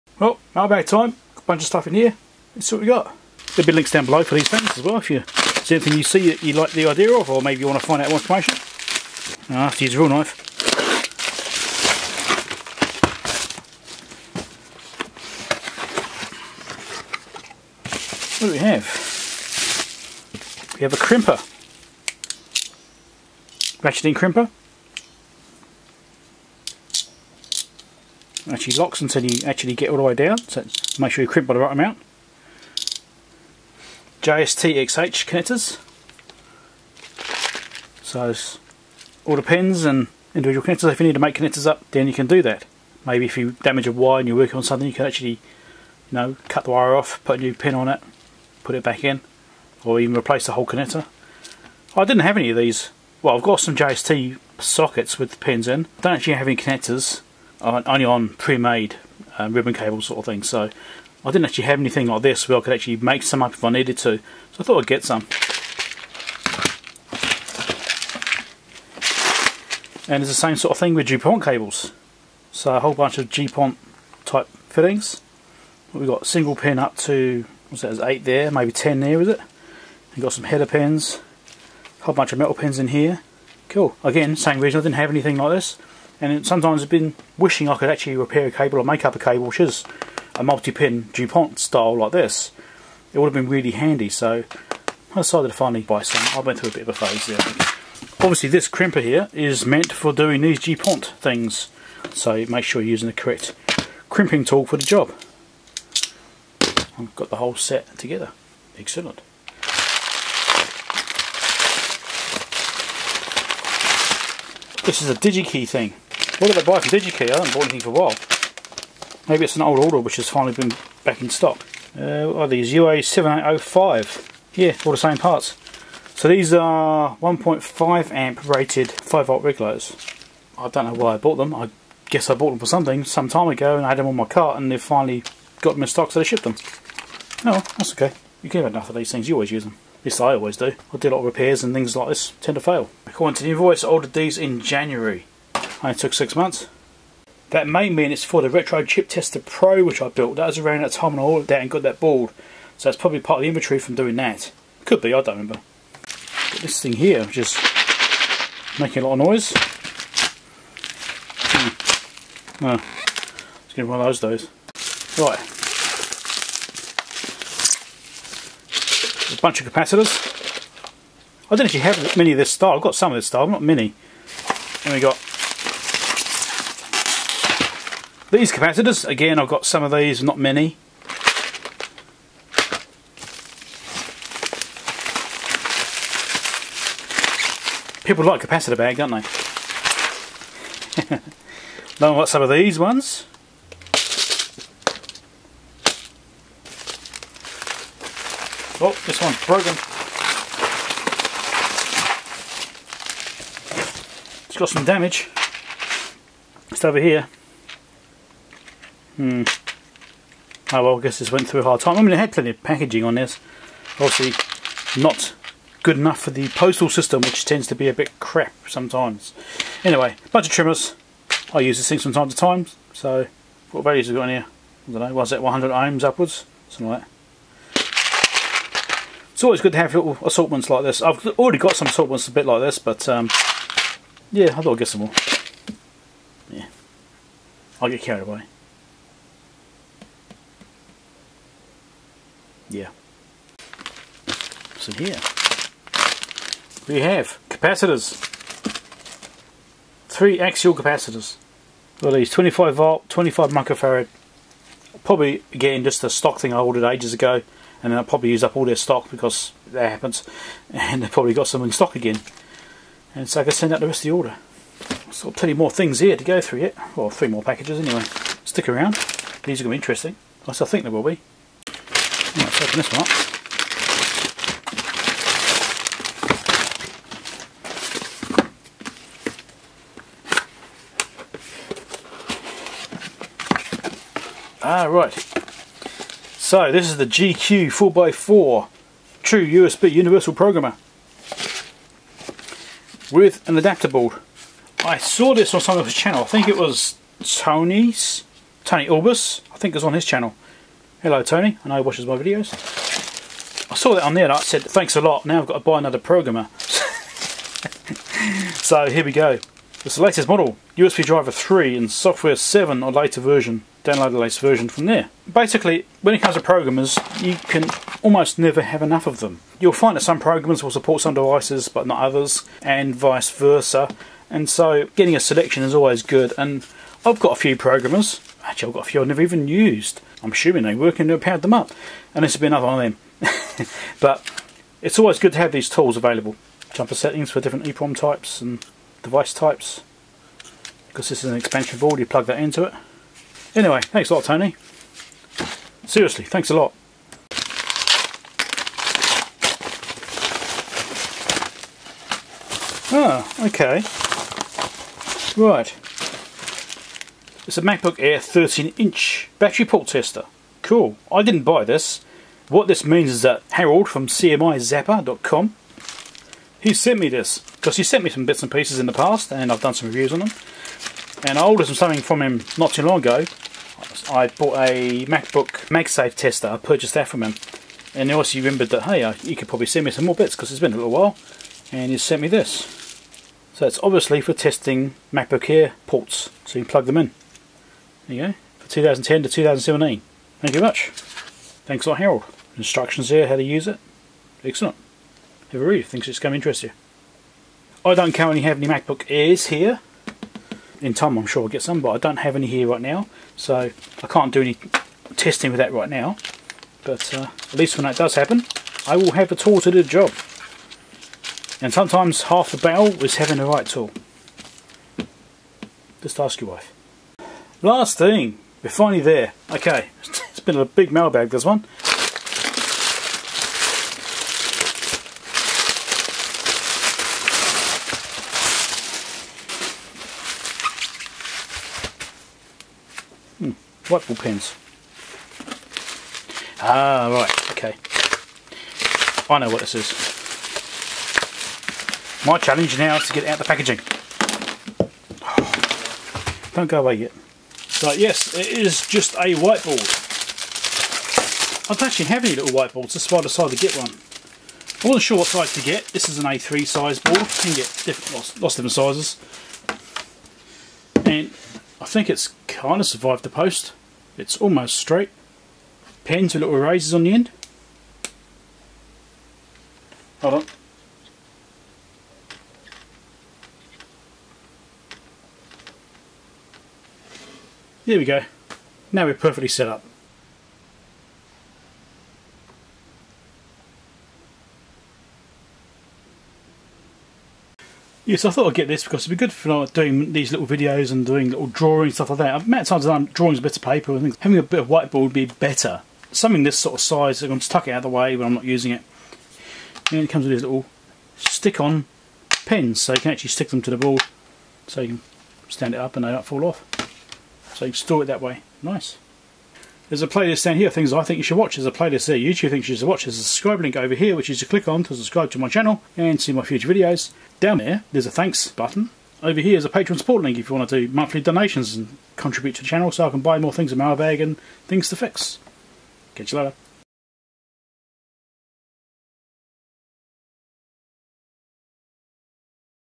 🔴 What's in my Mailbag ? - Mailbag Monday 25th July 2022 - No.1025
What's in my Mailbag ? - Mailbag Monday 25th July 2022

0:00 - Intro.
0:03 - Crimp and JST Connectors (no link).
1:50 - uA7805 5V regulators (no link).
2:35 - Plastic Film Capacitors
3:12 - Dipped Ceramic Capacitors
3:20 - Trimmers
4:04 - Axial Capacitors (no link).
4:24 - GQ-4x4 Universal Programmer
5:45 - CMIzapper MacBook Air Battery Port Tester
6:57 - Whiteboard and Pens (no link).
8:47 - Patreon Supporters

My Electronics Tools (video and purchase links)

HP/Agilent/Keysight
3457A.

Marconi
2022D.

Siglent
SDS1104X-E.
SPL-1008.